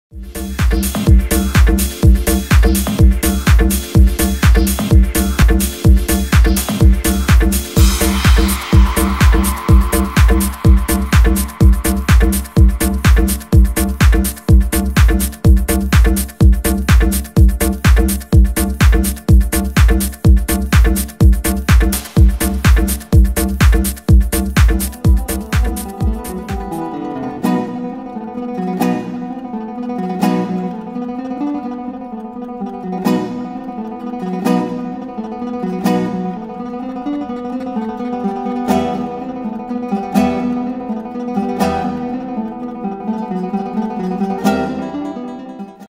Yellow Lounge Seoul - Milos & DJ Canisius (2012.05.02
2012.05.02 Club Ellui에서 Yellow Lounge 의 아시아 런칭!
훈남 클래식 기타리스트 Milos와 클래식 DJ 대명사 DJ Canisius 가 함께한 파티!!